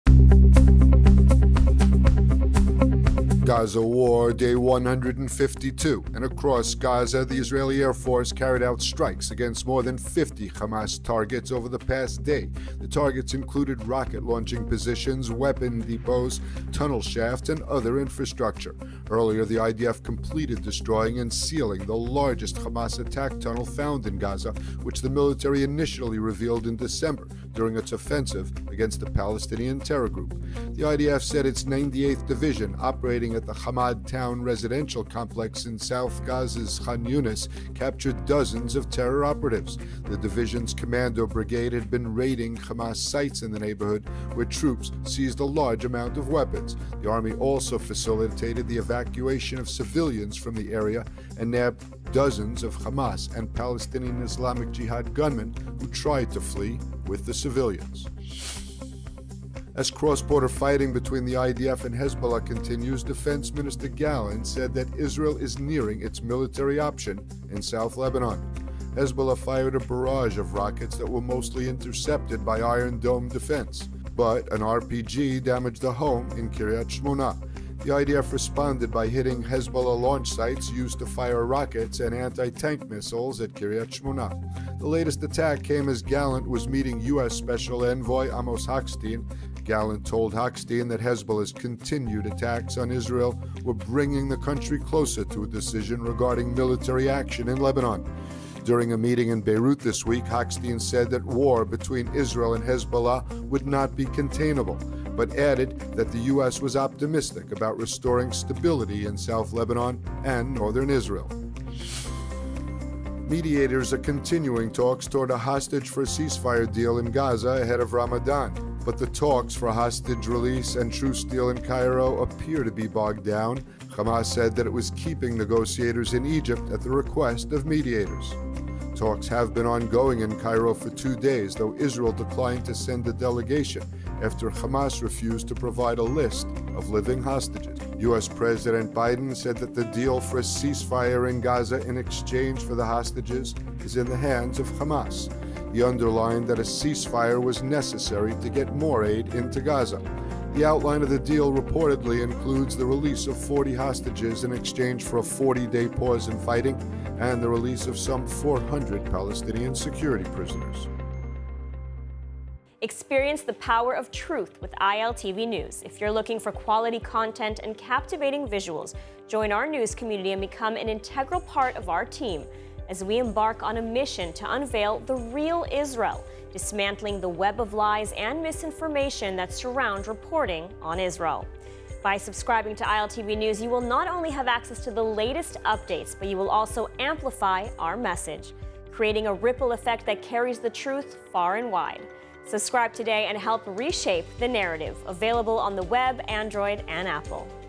ILTV News Flash - War Day 152, March 06, 2024
The Israeli Air Force carried out strikes against more than 50 Hamas. Cross-border fighting between the IDF and Hezbollah continues. And the talks for a hostage release and truce deal in Cairo seemingly bogged down